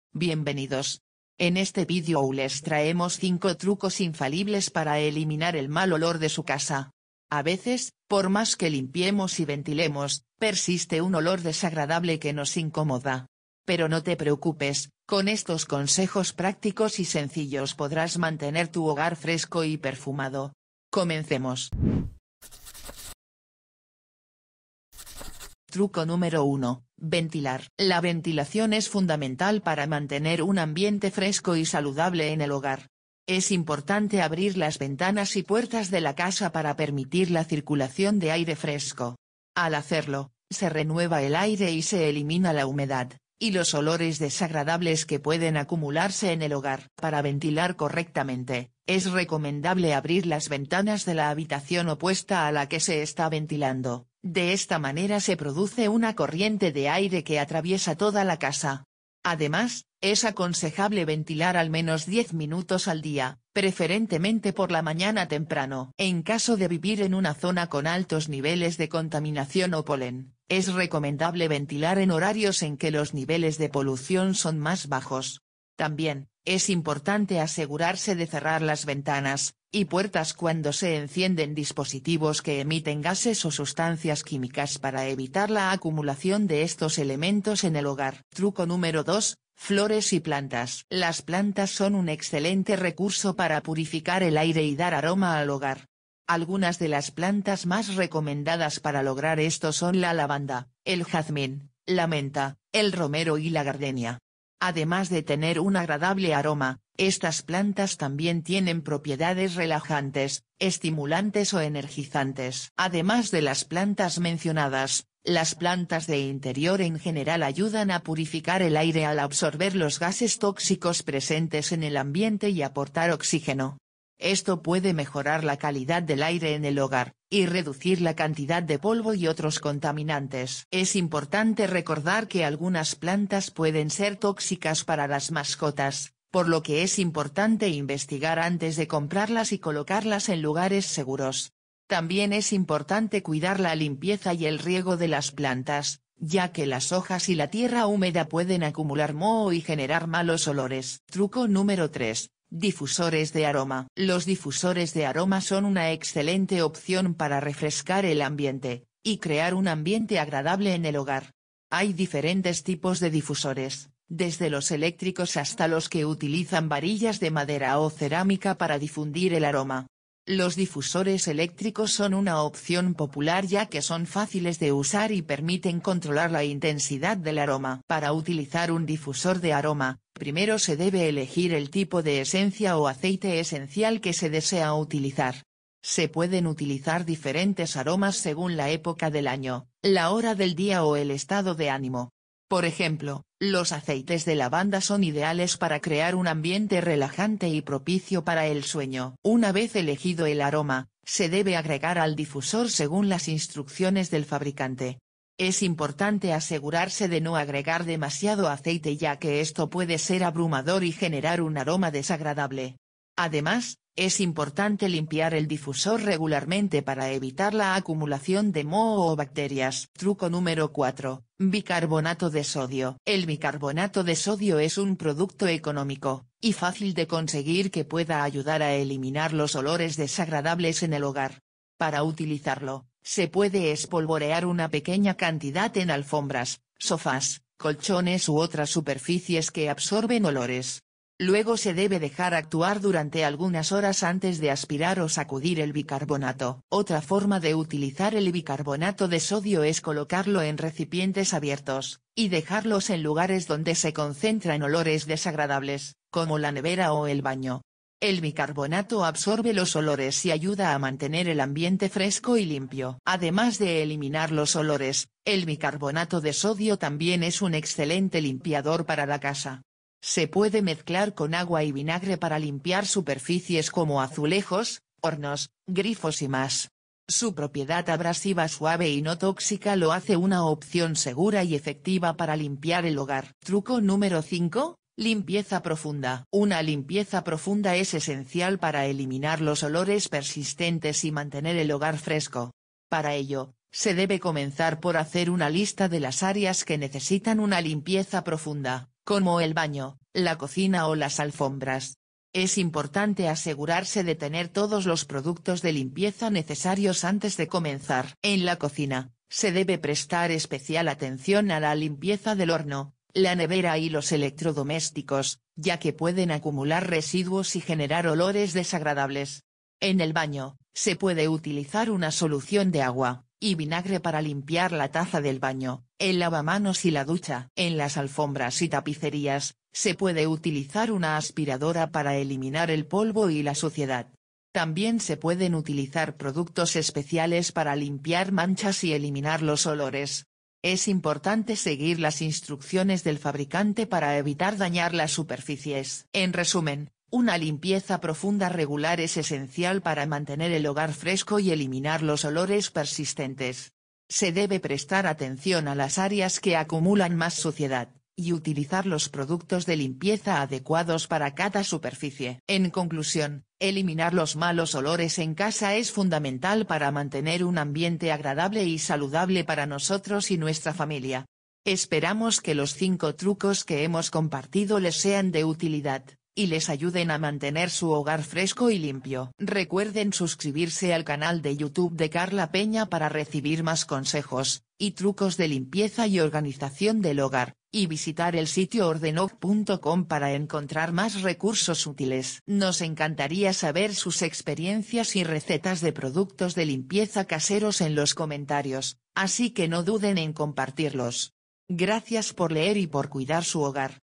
Elimina los olores desagradables de tu casa en minutos - 5 trucos para refrescar tu hogar
¡Bienvenidos! En este video les traemos 5 trucos infalibles para eliminar el mal olor de su casa. A veces, por más que limpiemos y ventilemos, persiste un olor desagradable que nos incomoda. Pero no te preocupes, con estos consejos prácticos y sencillos podrás mantener tu hogar fresco y perfumado. ¡Comencemos!

Truco #1: Ventilar. Se explica la importancia de ventilar y cómo hacerlo correctamente.
Truco #2: Flores y plantas. Se habla sobre las plantas que pueden ayudar a purificar el aire y dar aroma al hogar.
Truco #3: Difusores de aroma. Se explican los diferentes tipos de difusores y cómo utilizarlos para refrescar el ambiente.
Truco #4: Bicarbonato de sodio. Se detalla cómo utilizar bicarbonato de sodio para eliminar los olores desagradables.
Truco #5: Limpieza profunda. Se brinda información sobre cómo realizar una limpieza profunda para eliminar los olores y mantener el hogar fresco.